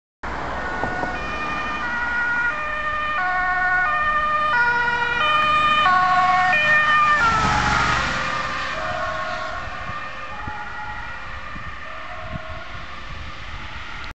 Ambulance 08-121 A1 Admiraalvlinderlaan Culemborg
09/08/09 Ambulance 21 is met spoed onderweg naar de Admiraalvlinderlaan in Culemborg